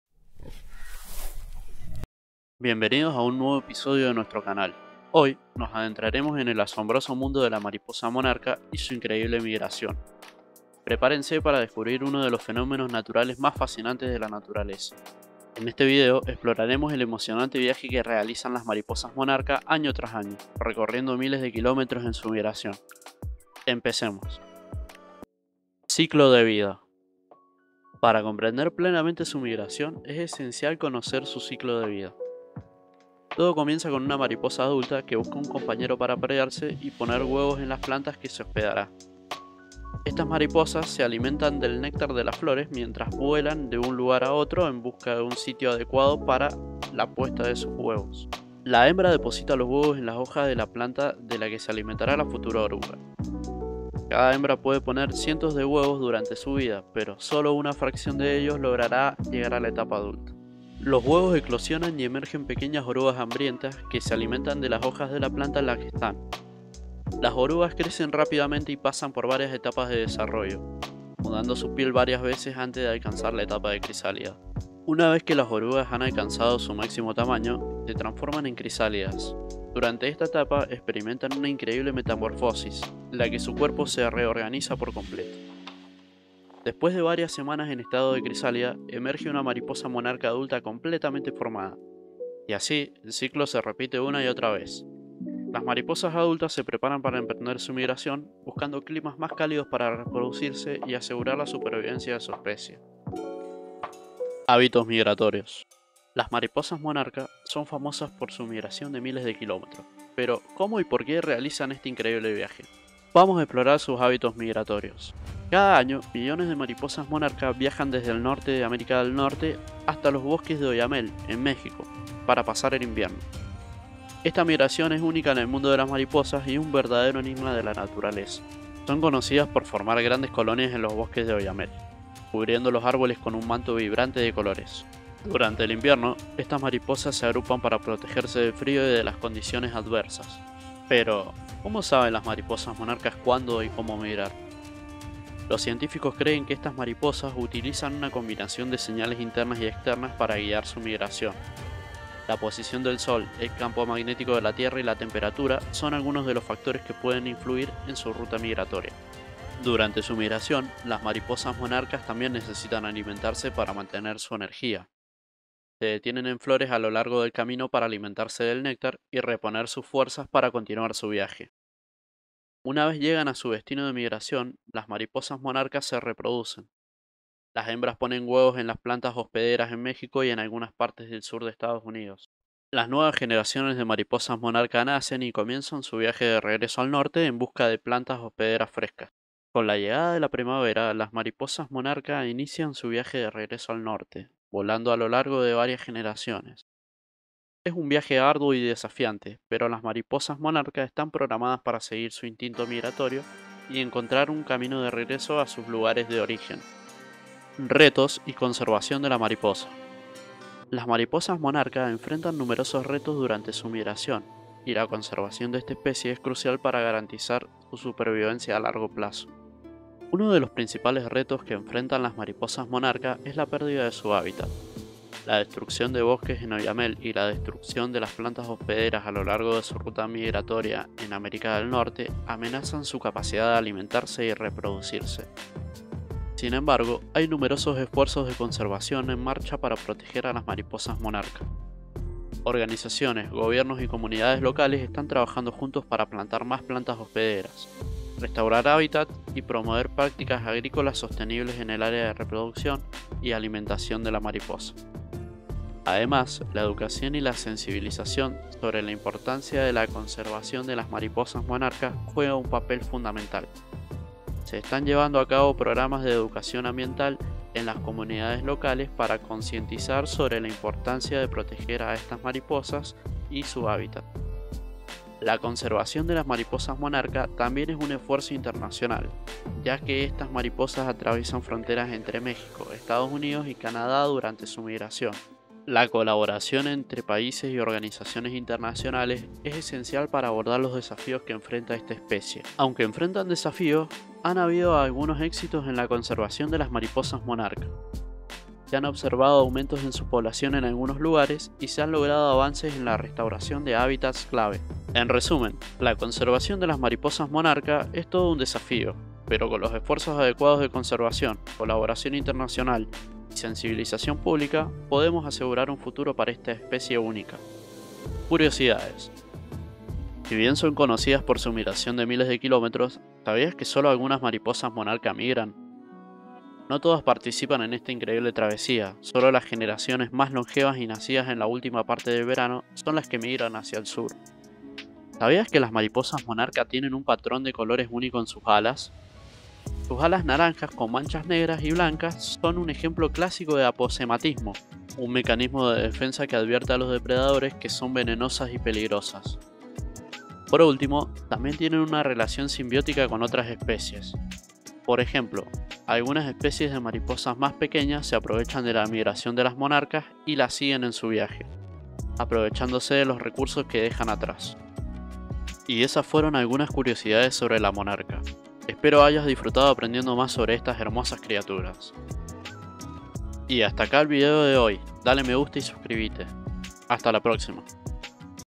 Mariposas Monarca: La migración más impresionante de la naturaleza
Descubre el increíble viaje migratorio de las mariposas monarca, una de las migraciones más asombrosas de la naturaleza. Acompáñanos en este emocionante viaje mientras exploramos los fascinantes detalles de cómo millones de mariposas monarca recorren miles de kilómetros en busca de su destino. Aprenderás sobre su ciclo de vida, sus hábitos migratorios, y los desafíos que enfrentan en su travesía. ¡Prepárate para maravillarte con este fenómeno natural único y sorprendente!"